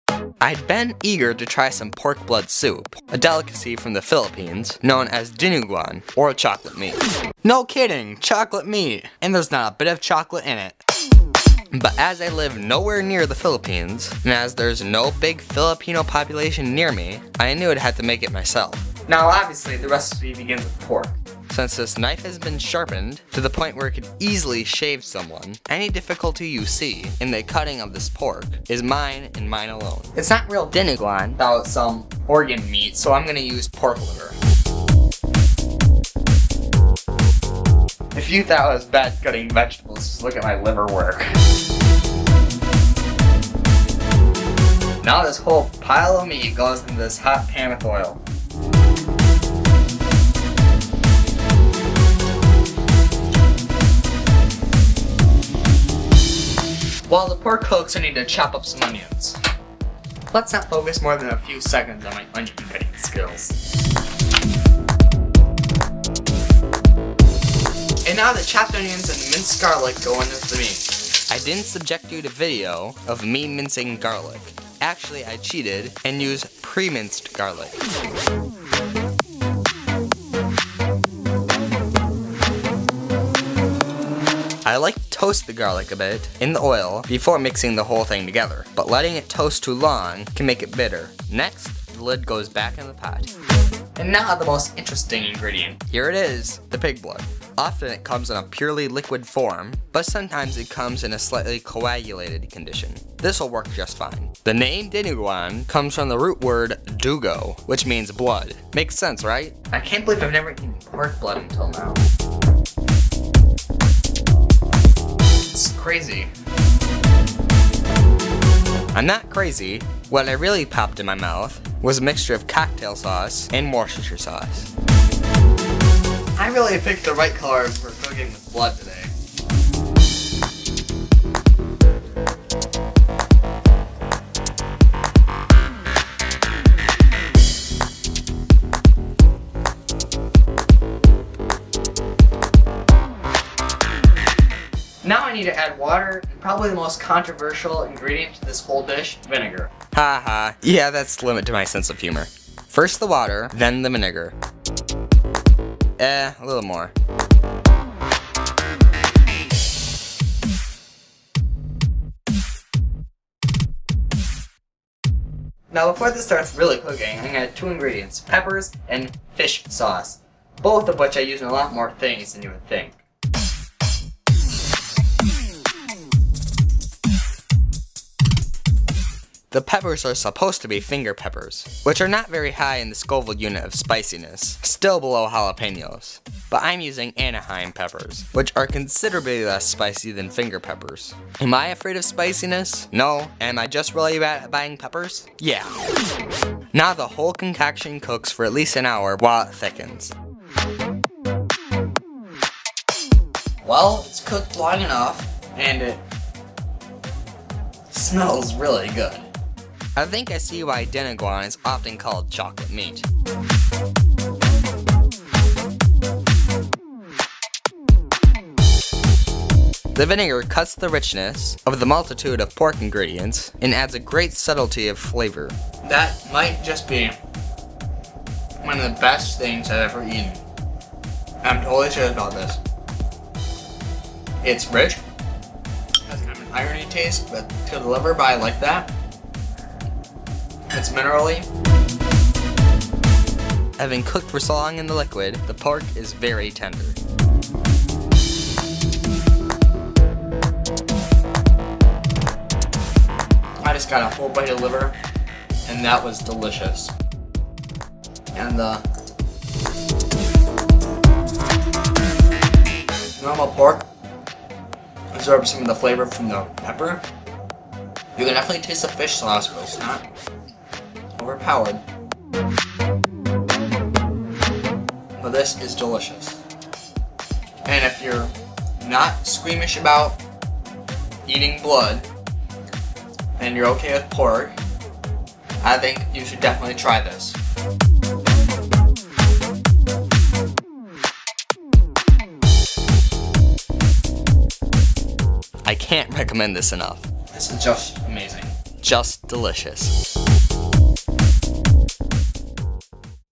Remy Eats Dinuguan / Pig Blood Stew
The ancient Spartans had a dish called Black Soup made of pork, vinegar and pig blood. Fast forward through the centuries and you'll find dishes in Europe and Asia that owe a debt of gratitude to the Greek dish.

But in the Philippines they've taken things a step further. Dinuguan, from the root word "dugo," the Filipino word for blood, adds organ meat and chiles to the ancient Greek meal and creates a dish that for some turns the stomach and for others induces fits of delight.